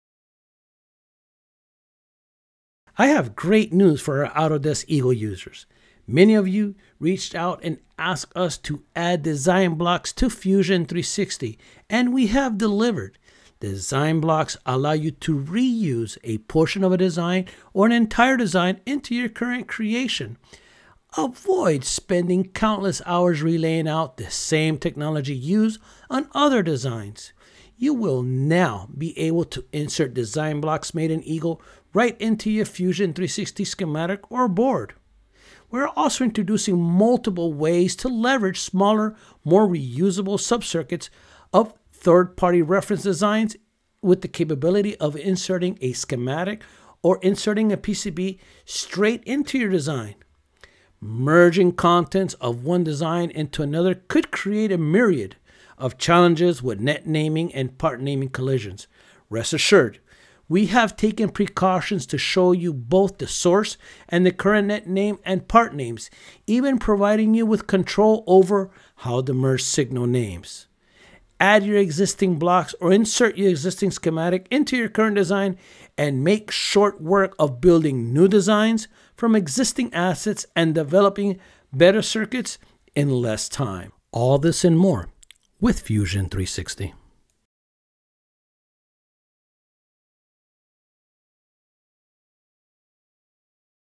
Design Blocks in Fusion 360 Electronics | Autodesk Fusion 360
Many Autodesk Fusion 360 users reached out and asked us to add Design Blocks, and we have delivered! Avoid spending countless hours repeatedly laying out the same technology used in other designs. Users are now able to insert design blocks made in EAGLE right into the Fusion 360 schematic or board. The team is introducing multiple ways to leverage smaller, more reusable sub-circuits or third-party reference designs.

Add existing Design Blocks or insert an existing schematic into a current design to quickly build a new design from existing assets and develop better circuits in less time.